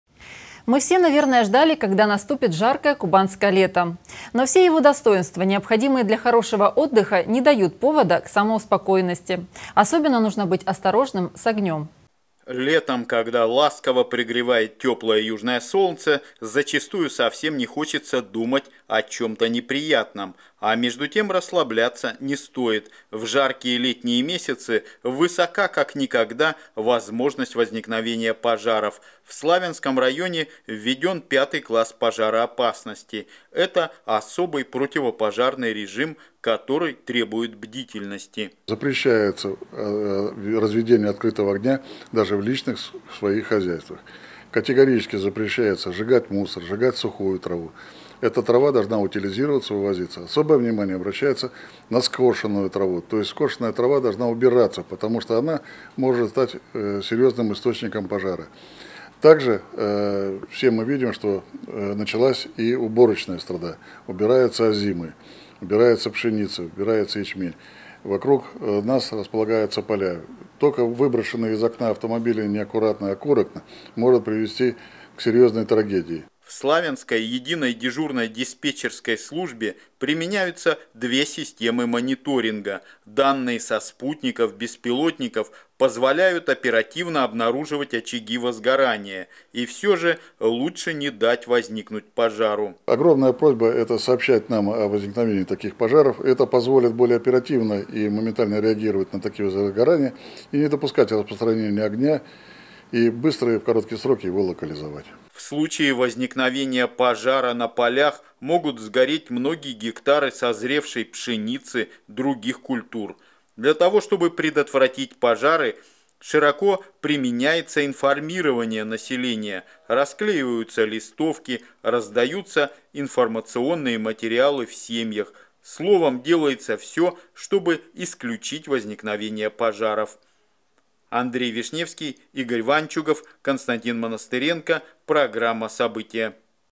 Будьте осторожны с огнем
Мы все, наверное,  ждали, когда насупит жаркое кубанское лето. Но все его достоинства, необходимые для хорошего отдыха, не дают повода к самоуспокоенности. Особенно нужно быть осторожным с огнем.